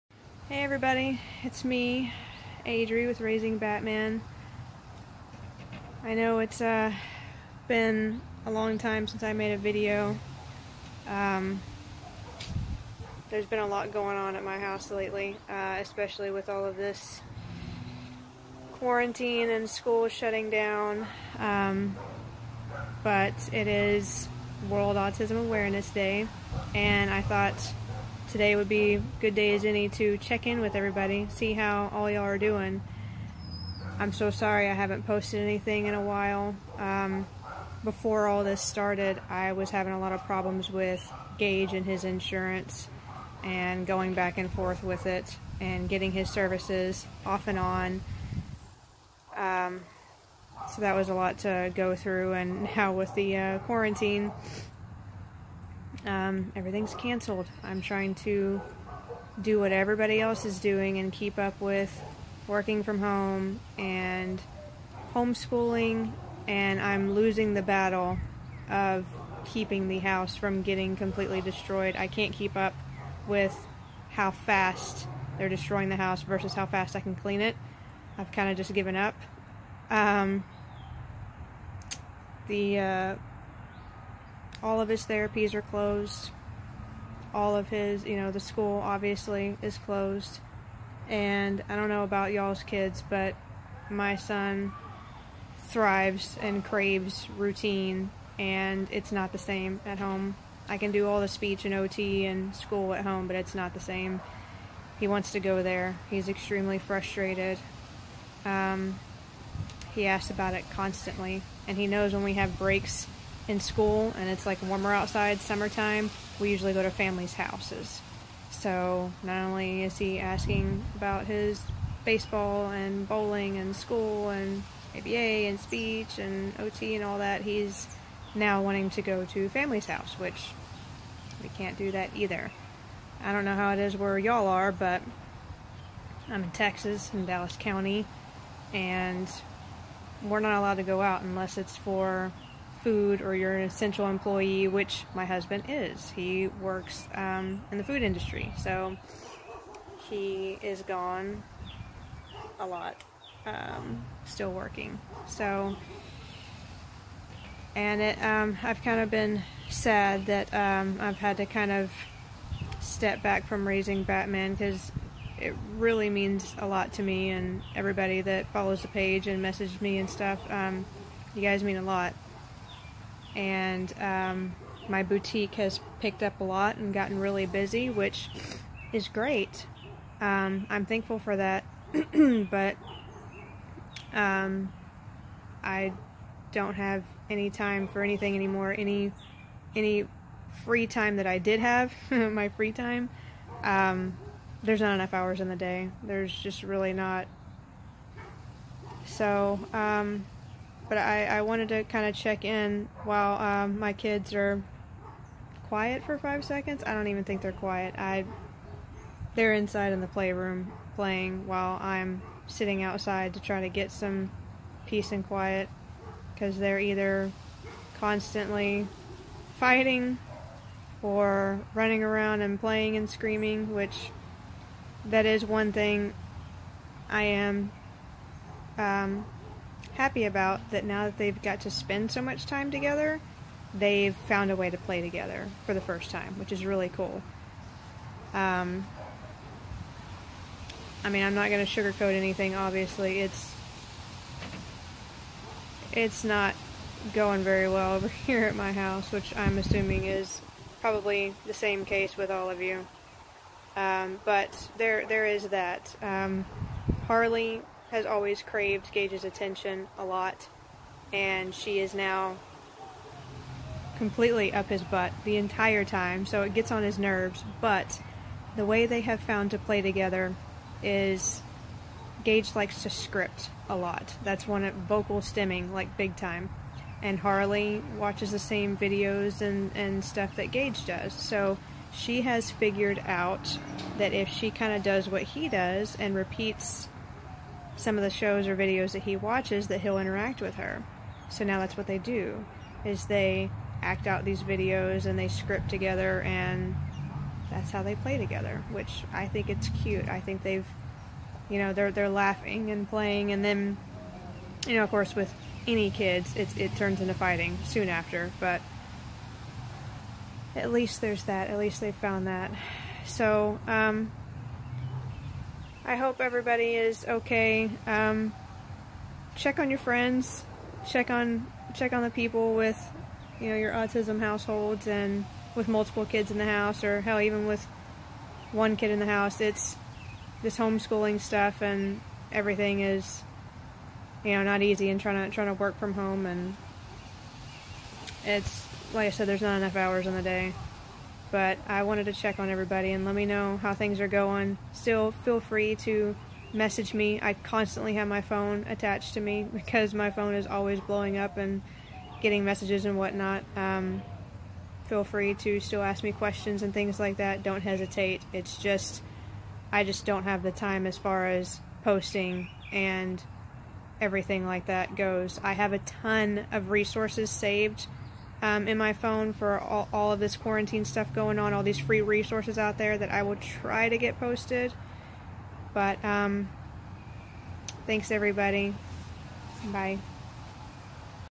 How is everyone doing?
Just an update since I haven't posted in awhile. It's been rough to say the least for the past few months for us...especially now with this quaranteen. Wanted to check and see how everyone else is doing 🙂